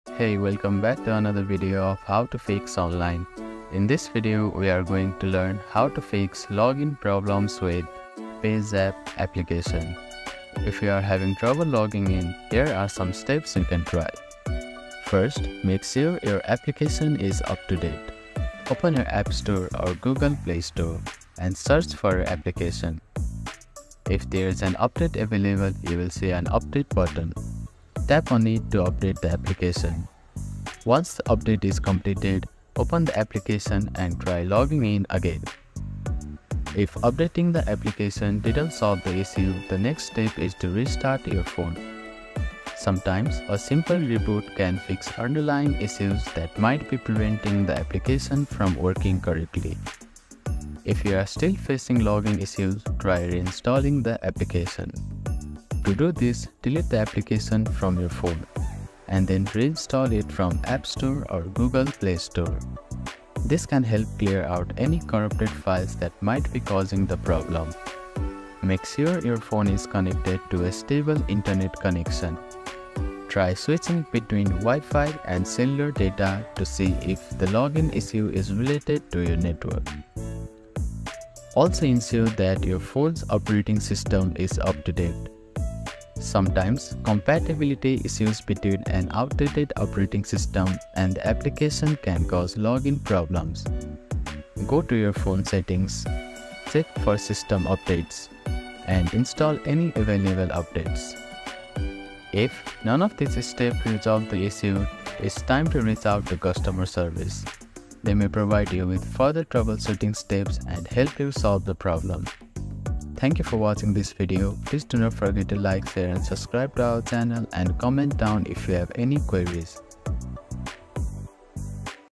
How to Fix Payzapp Account Login Not Working 2024?
Struggling to log in to your Payzapp account? You're not alone, and we're here to help! In this video, we'll walk you through the most common issues and their solutions so you can regain access to your Payzapp account quickly.